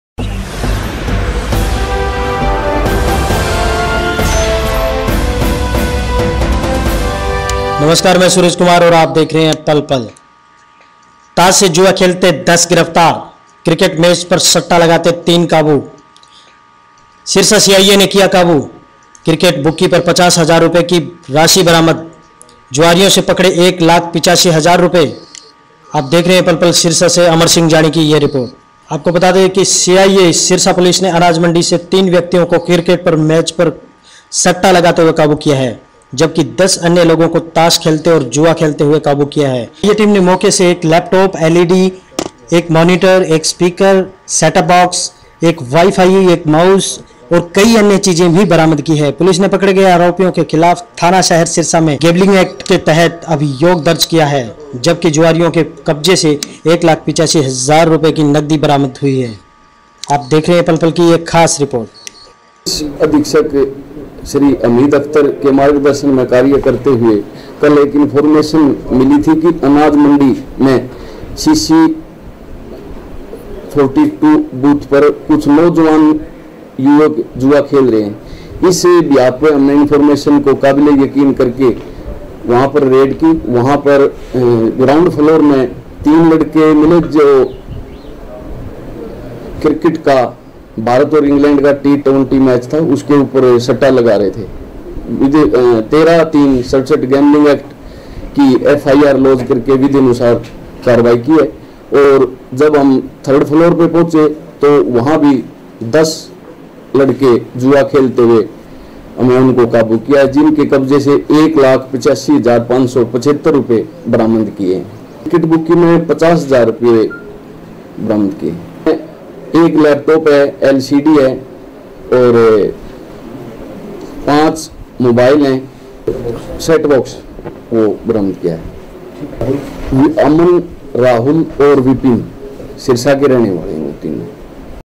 क्रिकेट बुक्की से 50000 रुपये की राशी बरामद-जुआरियों से पकड़े 1 लाख 85 हज़ार
सीआईए सिरसा  पुलिस ने अनाज मंडी सिरसा से 3 व्यक्तियों को
क्रिकेट मैच पर सट्टा लगाते हुए काबू किया। जबकि 10 अन्य लोगों को ताश से
जुआ लगाते काबू किया। सीआईए टीम ने मौका से एक लैपटॉप, एक LED, एक
मोनिटर, एक स्पीकर, एक सेट अप बॉक्स, एक बाई फाई, एक माउस, एक
कैलक्यूलेटर, 2 रिमोट, 5 मोबाइल फोन व 50 हज़ार रुपयों की नगदी बरामद की
है ।  पुलिस ने पकड़े गए आरोपीयों के खिलाफ थाना शहर सिरसा में गैंबलिंग
एक्ट के तहत अभियोग दर्ज किया है। जबकि जुआरियों के कब्जे से 1 लाख 85
हज़ार की नगदी बरामद हुई है।
सीआईए सिरसा इंचार्ज अजय कुमार की एक पुलिस टीम ने अनाज मंडी
सिरसा के नजदीक मौजूद थी की गुप्त सूचना के आधार पर अनाज मंडी के बूथ
नंबर CC 42 पर छापा मारकर आरोपी विपिन बंसल, राहुल मित्तल व पवन बत्रा
उपरोक्त को भारत व इंग्लैंड के बीच चल रहे T 20 क्रिकेट मैच के ऊपर
इन्टरनेट व टीवी के माध्यम से सट्टा खेलते हुए 50 हजार रुपयों की राशी
सहित काबू कर लिया आरोपीयों के खिलाफ मुकदमा नंबर  दिनांक 08.7.18 धारा
13A.3.67 गैंबलिंग एक्ट थाना शहर सिरसा दर्ज करके आगामी कारवाही अमल में
लाई जा रही है । दूसरे मामले में ताश से जुआ खेलते 10 लोजी गिरफ्तार किए
गए।